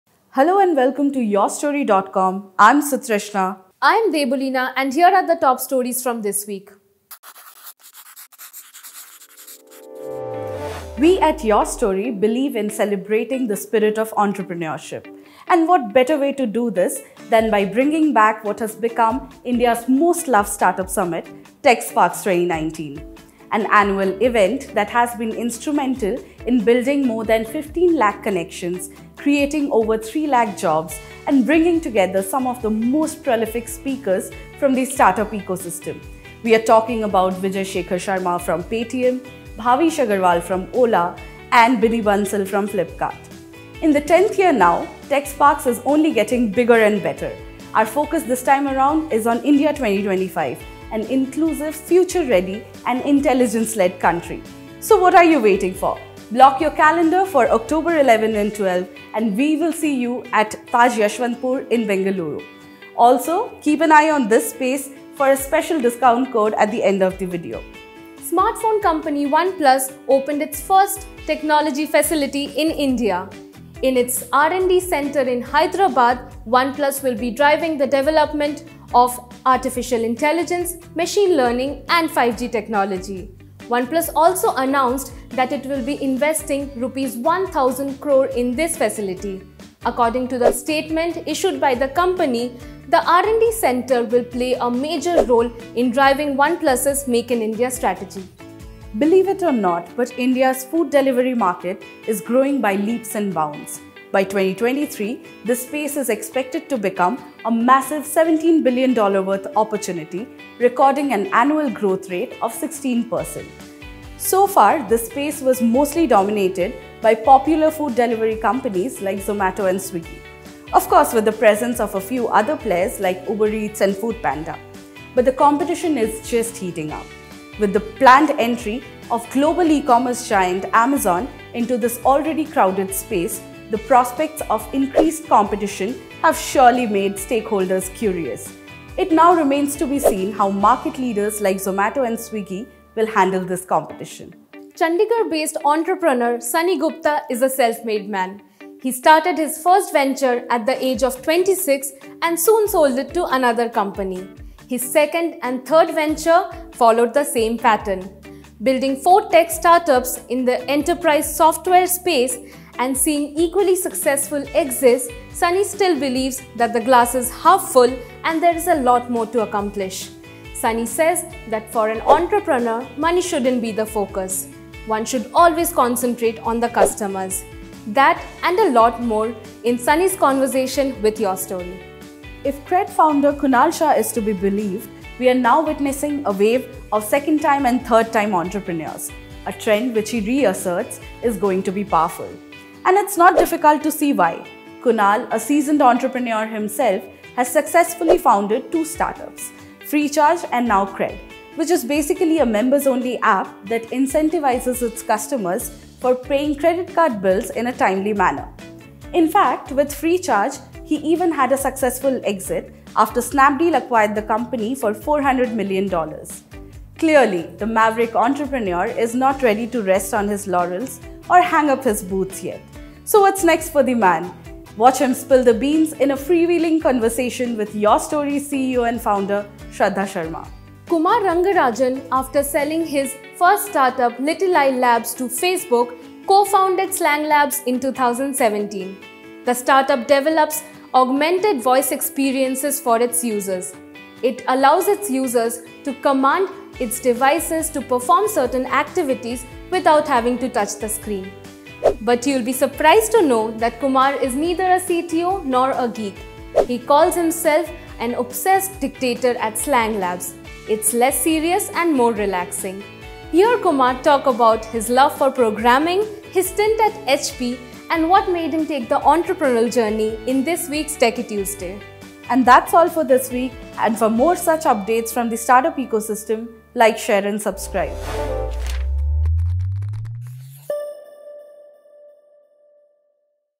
Weekly Wrap - Top Stories of the Week - Aug 30th
This week, we have some news or our own, and more about the startup ecosystem. From in-depth conversations with Kunal Shah of Cred and Ankush Aggarwal of Avail Finance to Indian foodtech’s latest battle.

Hosts: Sutrishna Ghosh & Debolina Biswas
Team: Aishik Purkait & Shlok Bhatt